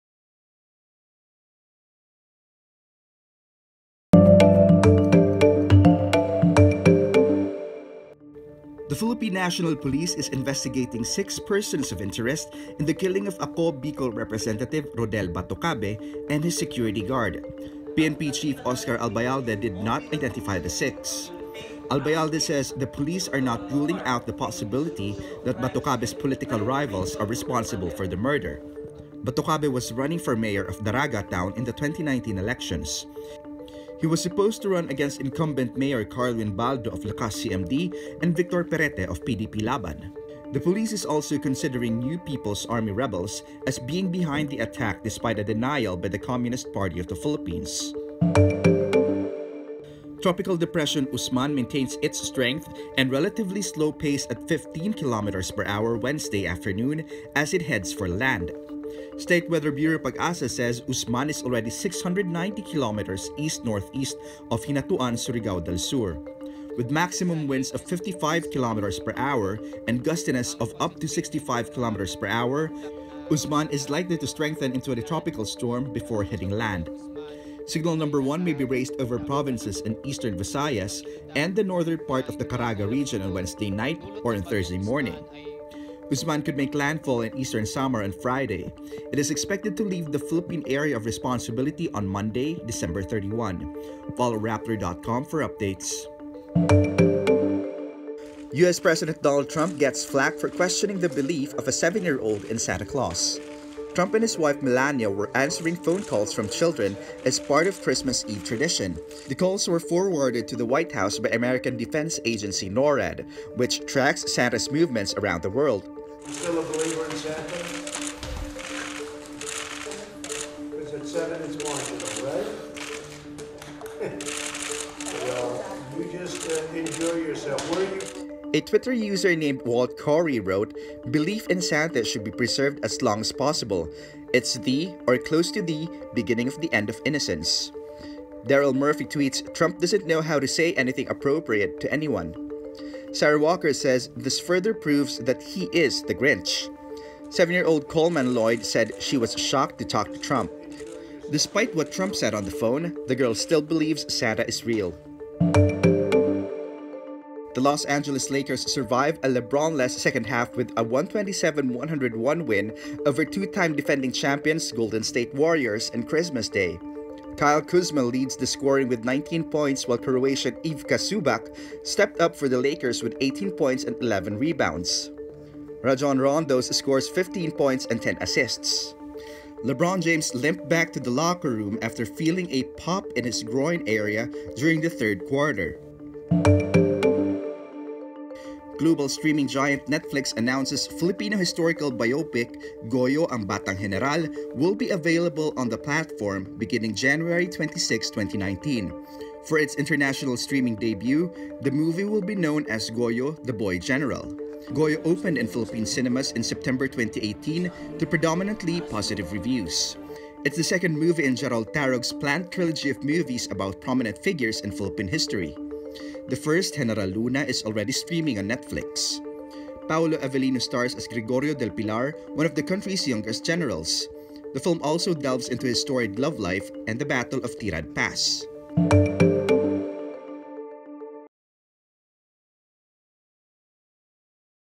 PNP identifies 6 persons of interest in Batocabe murder | Evening wRap
- PNP looking into 6 persons of interest in Batocabe slay
- Rainy last week of 2018 expected due to Usman
- On Christmas Eve, Trump questions child about belief in Santa
- Without LeBron, Lakers trounce Warriors on Christmas Day
- ‘Goyo: Ang Batang Heneral’ will be streaming on Netflix in 2019 Follow Rappler for the latest news in the Philippines and around the world.